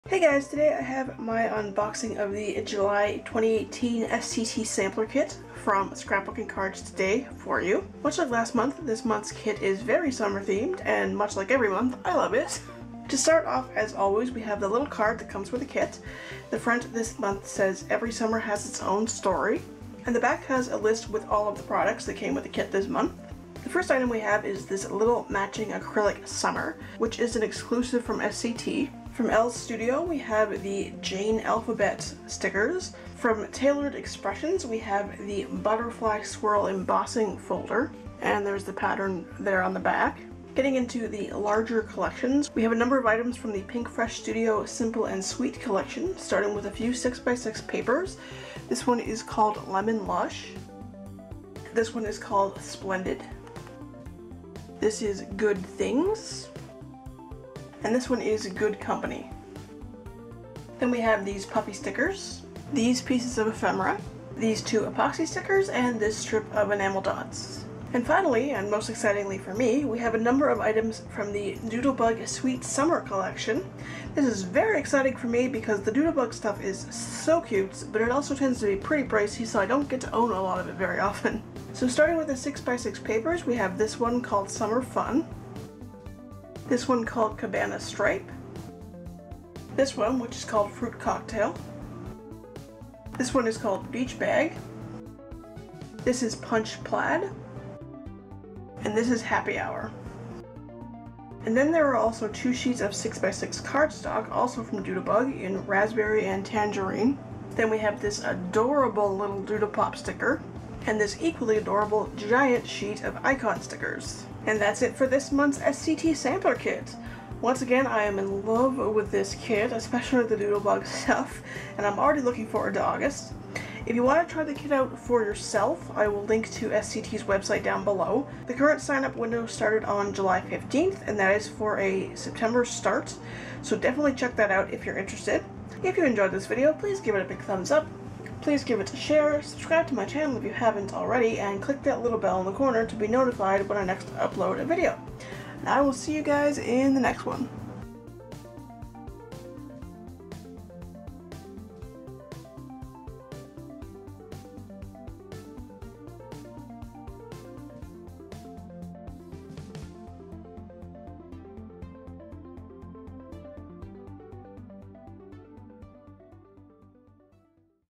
UNBOXING: The SCT Sampler Kit for July 2018!
NOTE: Captions are fixed! :-)

This month's SCT Sampler Kit is here! Featuring items from Doodlebug, Pinkfresh Studio, Taylored Expressions, and more!

This video is closed captioned! (By me, not YouTube!) Click the "CC" icon to use them :-)